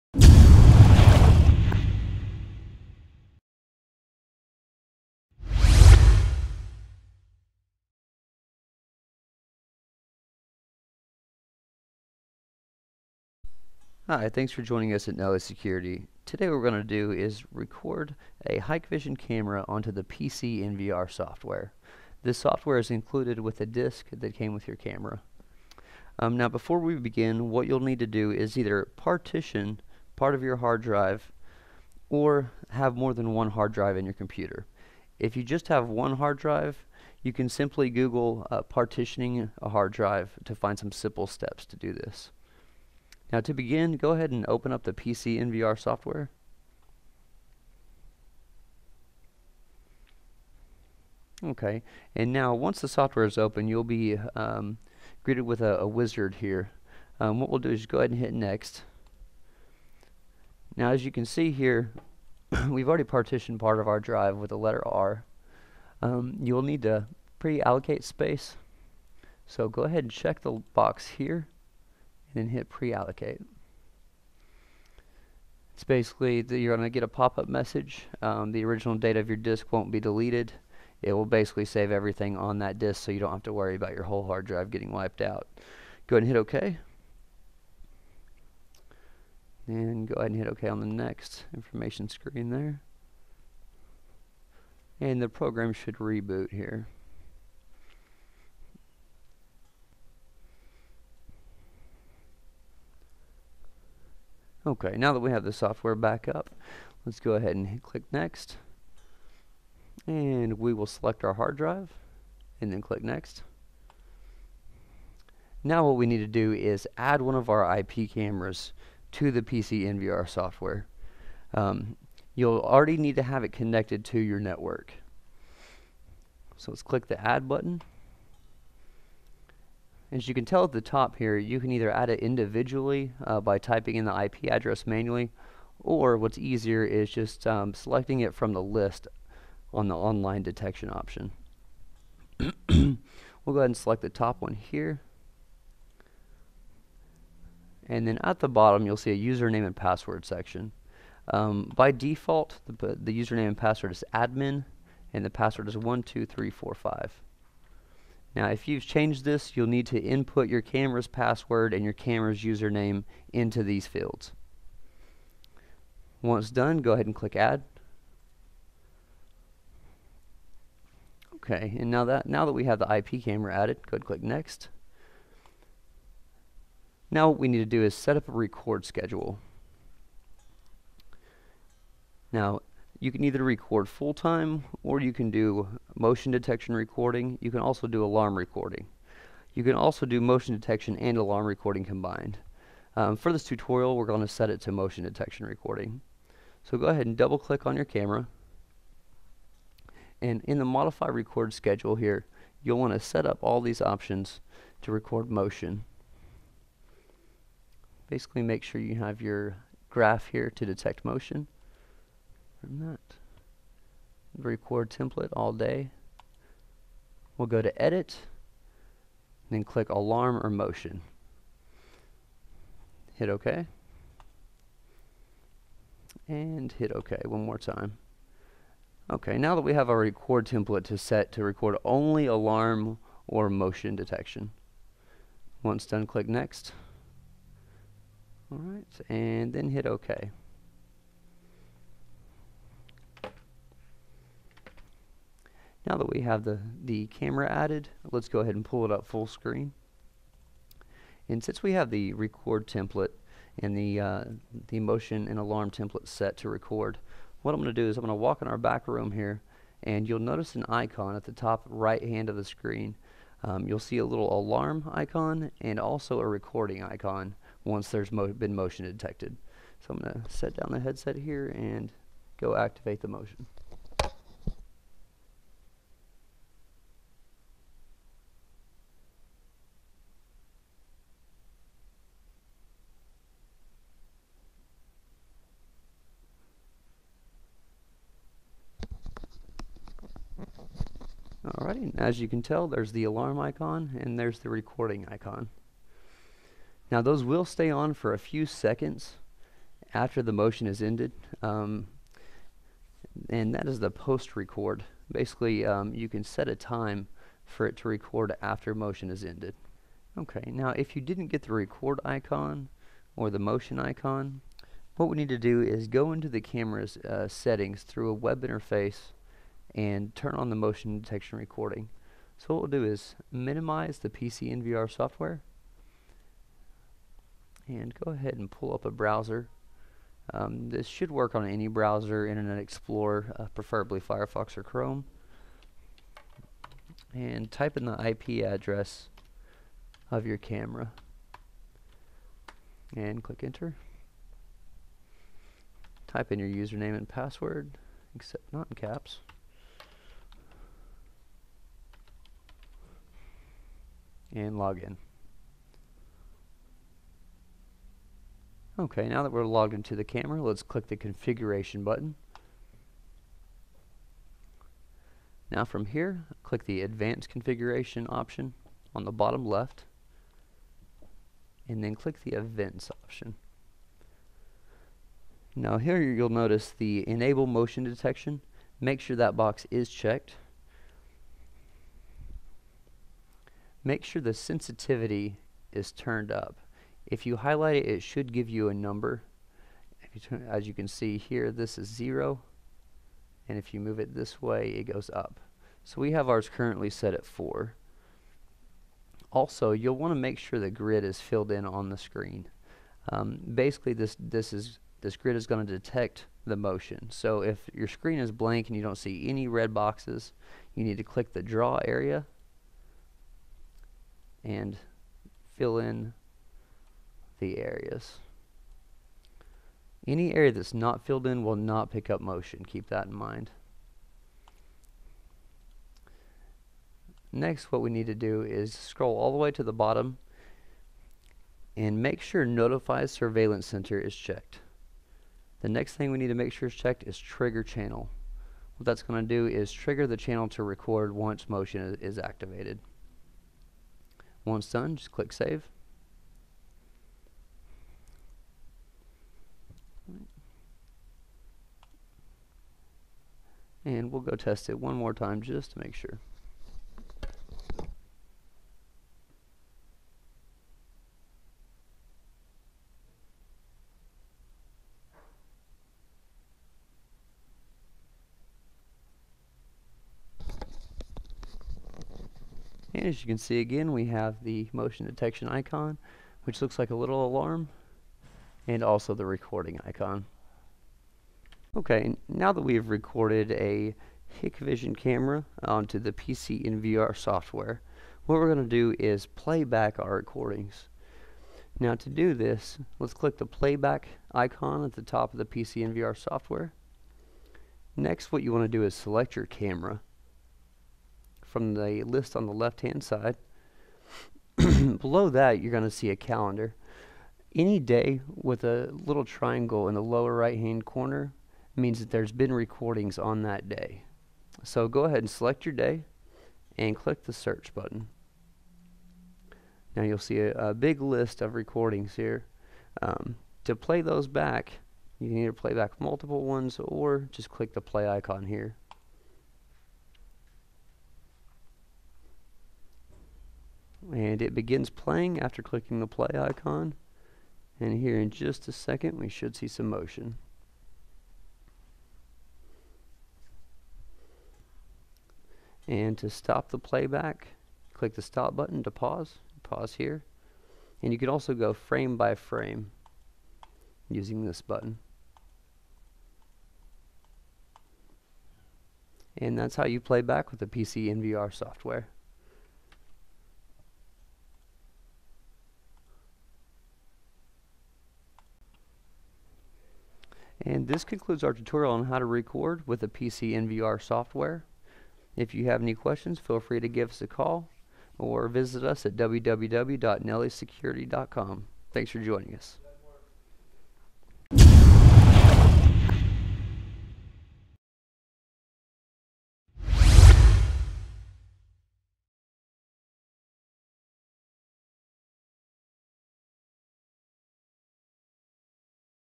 How to record a Hikvision IP Cam using iVMS-4200 PCNVR (v1.03)
This video tutorial takes you through each step to record a Hikvision IP camera using the iVMS-4200 PCNVR software. We no longer carry Hikvision products. to view our current product lines.